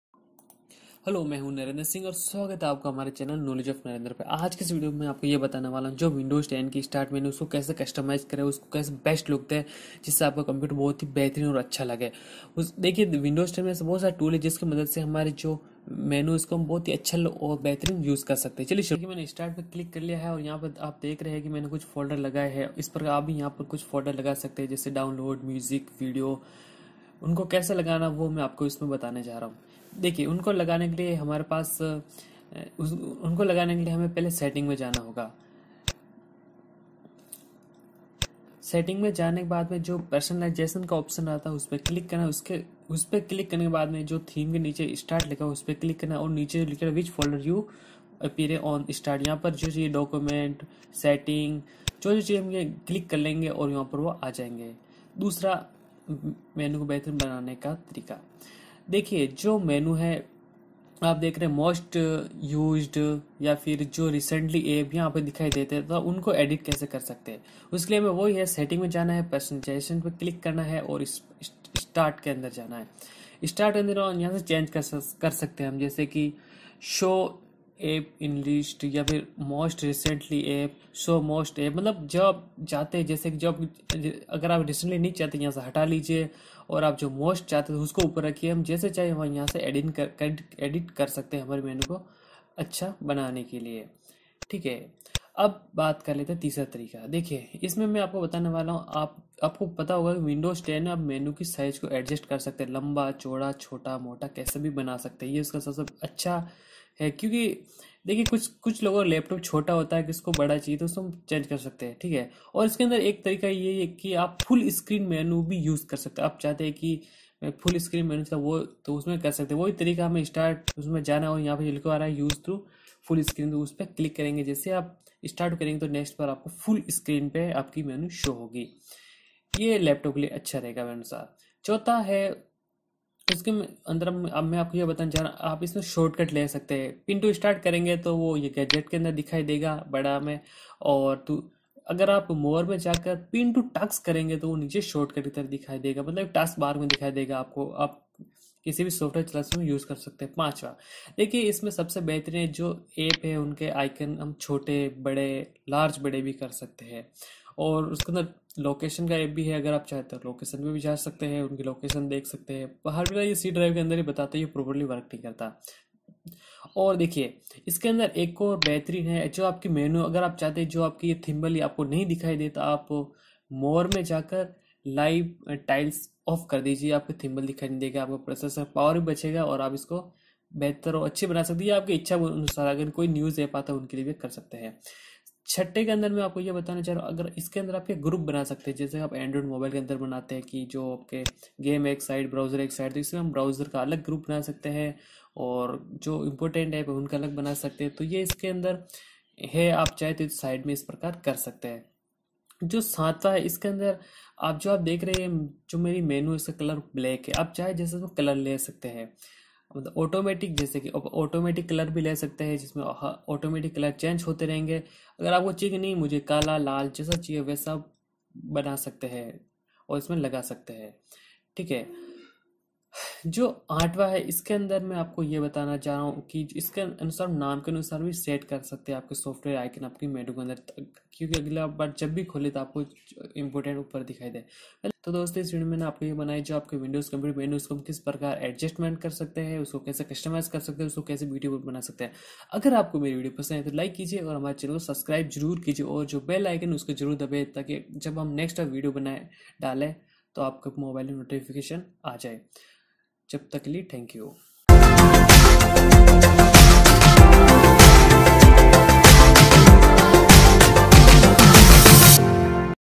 Customize The Windows 10 Start menu...!
How To Customize Start Menu In Windows 10. And Make Beautiful Start  Menu. Edit Icon Size. Learn Personalisation Setting in Windows 10.